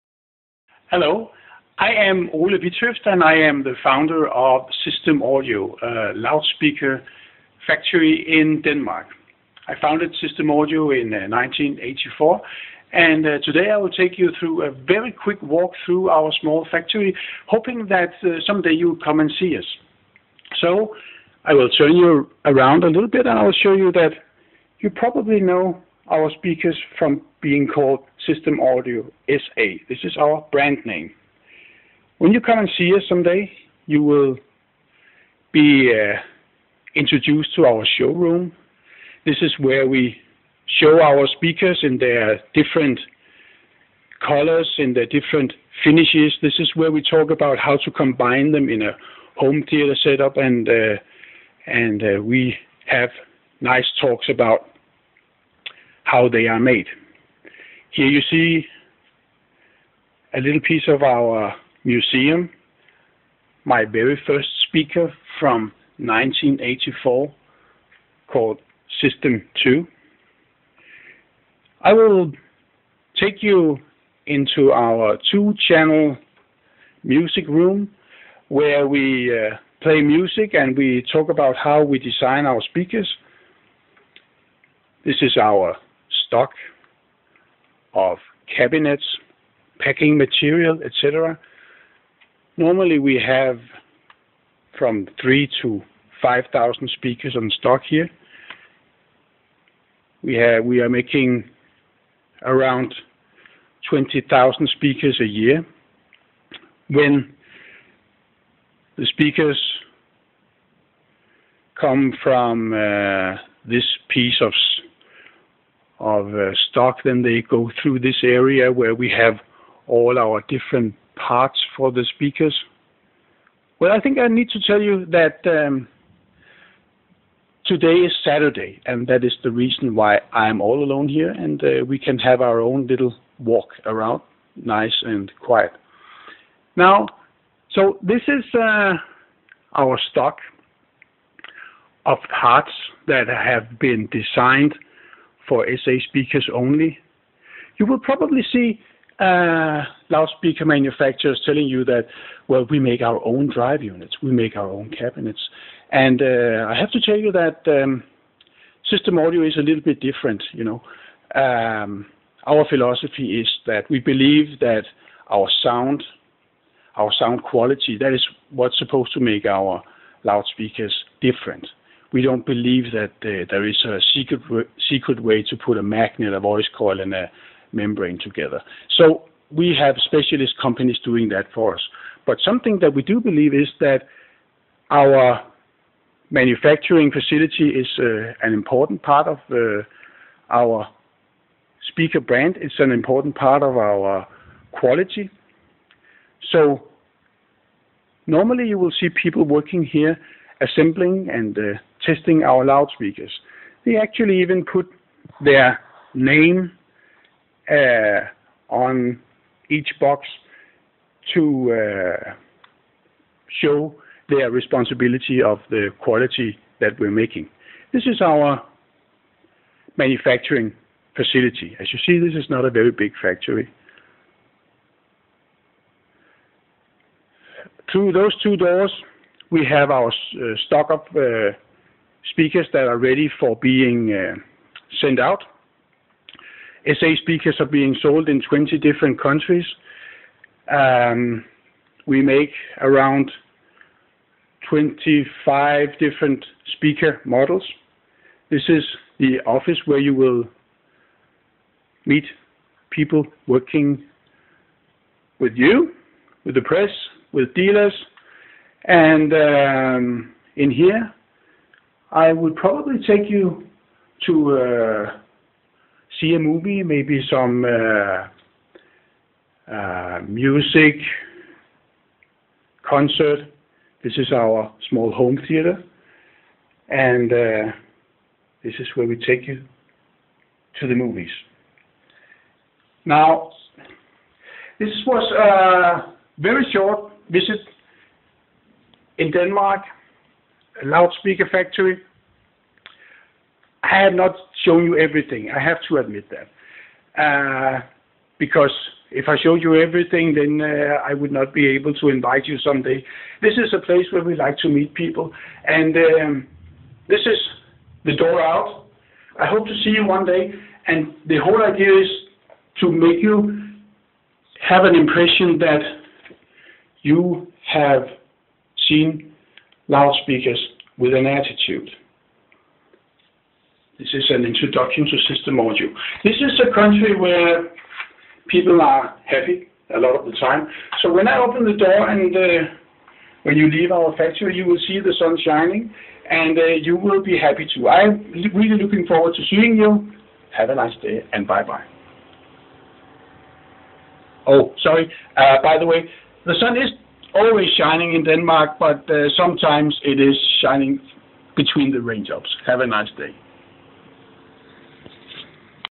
FACTORY TOUR at System Audio (English)
Spontanous, 6 minute walk through the 1500 sqm. SA factory with Ole Witthøft on a Saturday.